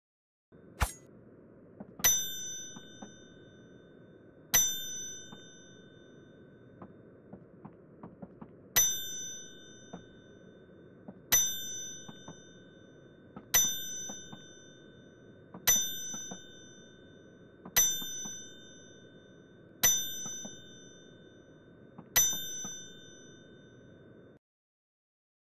I found a thing
I found a working scale in arena mode in MPN
I have been doing this for like 10 minutes